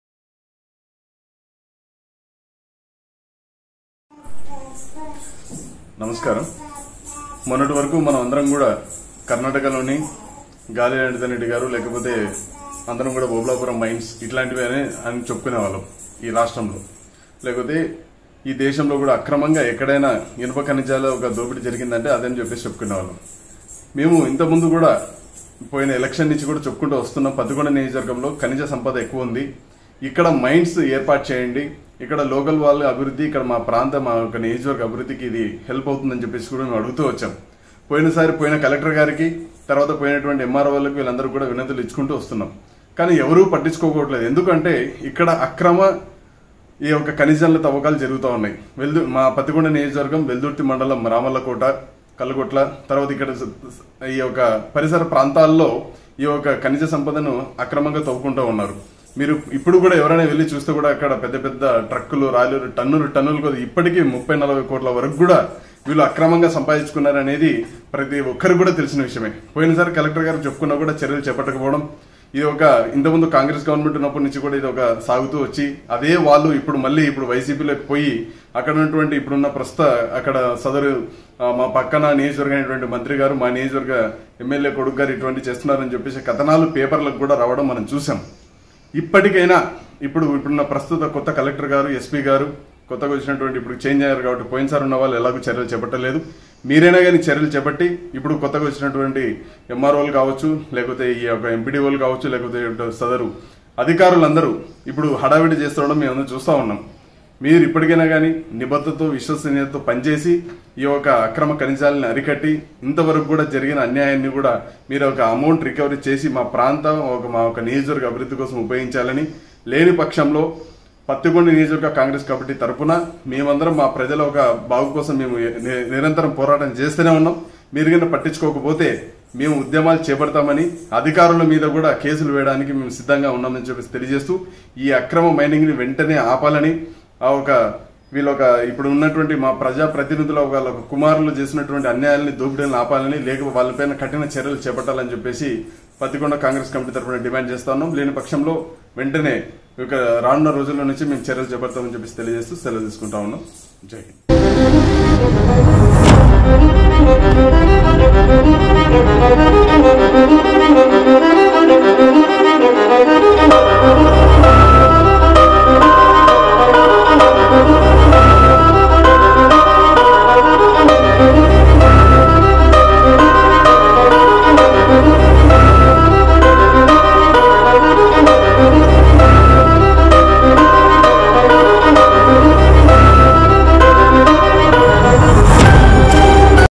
మన నియోజకవర్గం - మన హక్కు - మన బాధ్యత
The LedhuNiyojakavargamofAP పత్తికొండ నియోజకవర్గంలో అడుగడుగునా మోసం, అన్యాయం, అక్రమాలు, దోపిడీ, దౌర్జన్యం, కబ్జాలు, దోచుకోవడం.

మన నియోజకవర్గంలో ఎన్నో సమస్యలు, ప్రాథమిక హక్కులు అయిన "విద్య, ఉపాధి, పరిశ్రమ, ఆరోగ్యం మరియు నీరు" కోసం పోరాడుతూనే ఉన్నాం.

స్థానికులను ఎన్నుకుందాం, మన నియోజకవర్గాన్ని అభివృద్ధి మరియు సాధికారత దిశగా నడిపించుకుందాం..!!

జై హింద్.

క్రాంతి నాయుడు
పత్తికొండ నియోజకవర్గ కాంగ్రెస్ పార్టీ ఇంచార్జి.